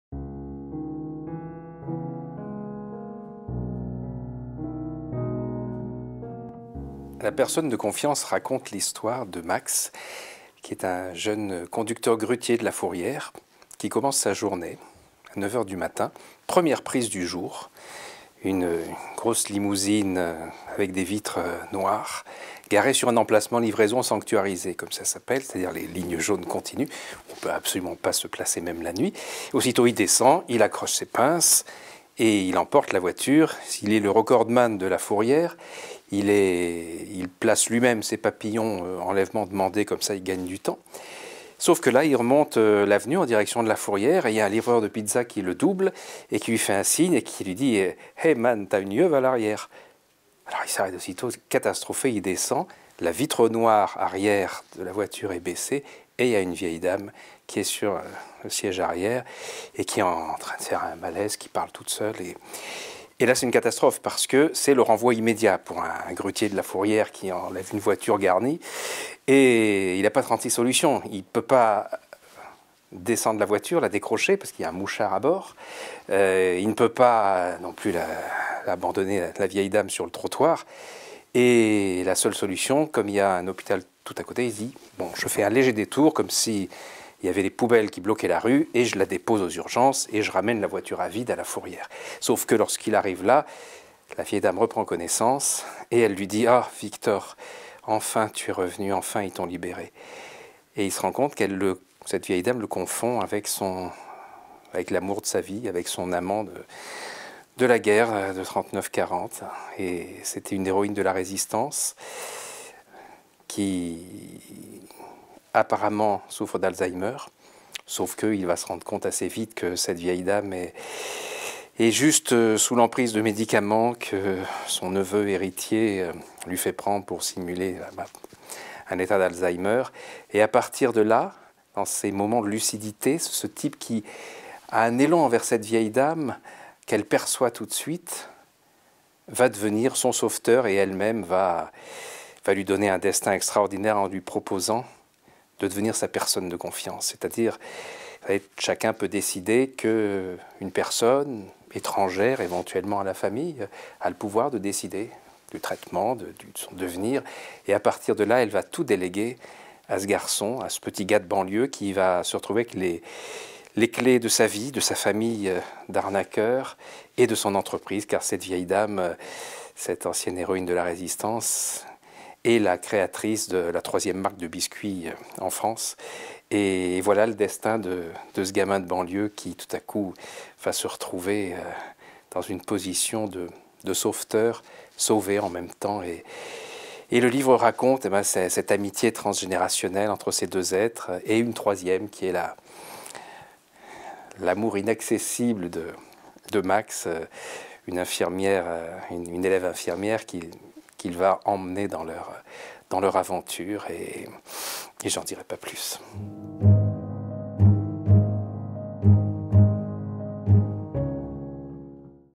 Didier Van Cauwelaert - La personne de confiance
Didier Van Cauwelaert vous présente son ouvrage "La personne de confiance" aux éditions Albin Michel.